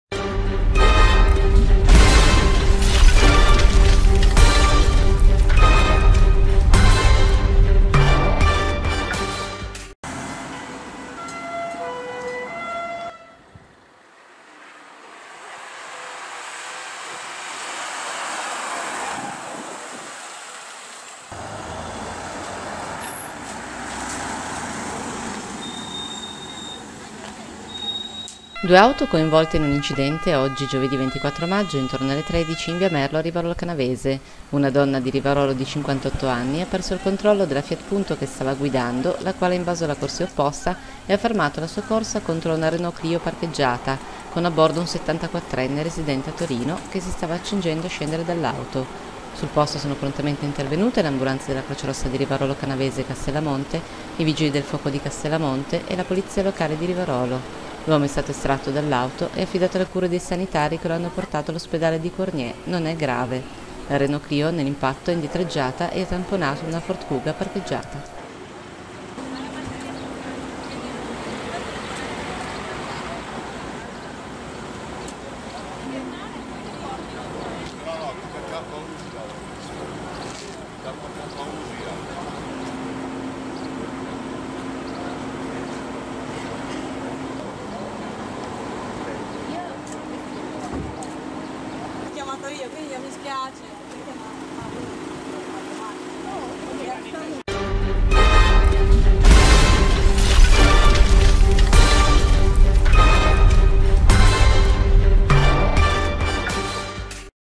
Rivarolo Canavese - Incidente in Via Merlo
Rivarolo Canavese - Incidente in Via Merlo: intervento della CRI, Vigili del Fuoco e Polizia Municipale